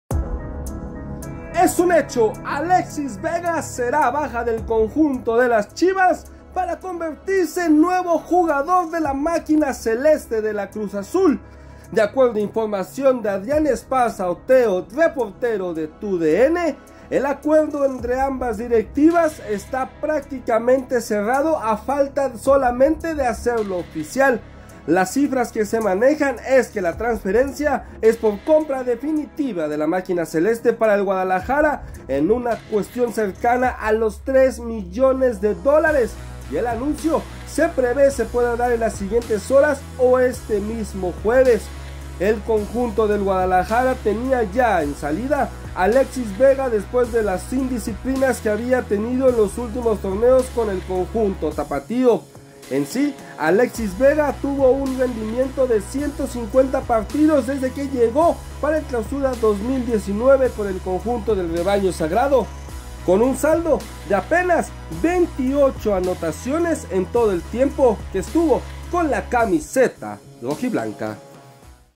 🚨💣 ¡NUEVO REFUERZO! 🚂🔵 Alexis Vega, muy cerca de llegar a Cruz Azul | TUDN
Con información de Adrián Esparza, es prácticamente un hecho que Alexis Vega se sumará a las filas de Cruz Azul, de cara al Clausura 2024.